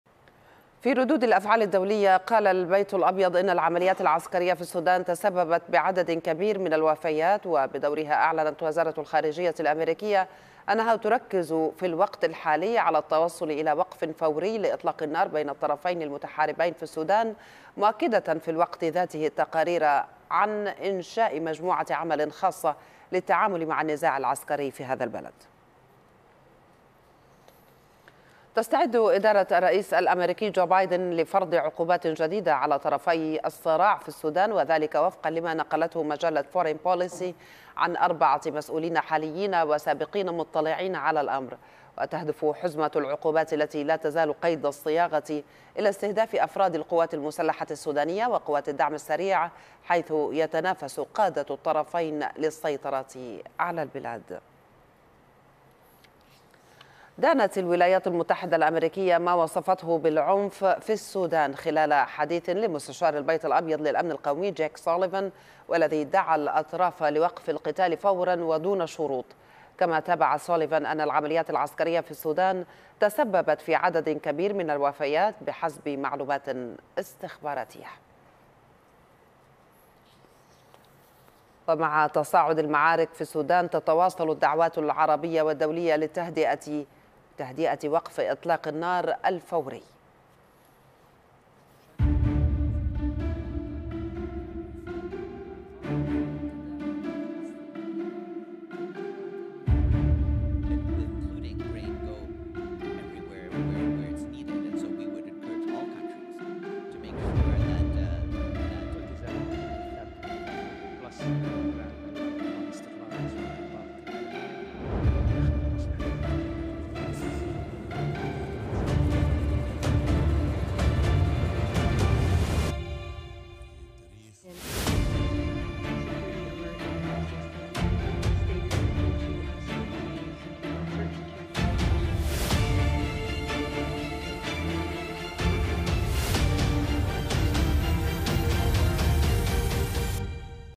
أميركا: القتال في السودان تسبب بالكثير من الوفيات وسنفرض العقوبات على أطراف النزاع - أخبار الشرق
قالت المتحدثة باسم البيت الأبيض كارين جان بيير، الأربعاء، إن "الولايات المتحدة تدين العنف في السودان"، مشيرة إلى أن "العمليات العسكرية تسببت في عدد كبير من الوفيات".
وأدانت سفارات دول غربية، الأربعاء، بأشد العبارات استمرار العنف في السودان، بما في ذلك الهجمات الموجهة ضد المدنيين والدبلوماسيين والجهات الفاعلة الإنسانية، بين القوات المسلحة السودانية وقوات الدعم السريع، ودعت الطرفين إلى إنهاء الأعمال العدائية على الفور ودون شروط مسبقة.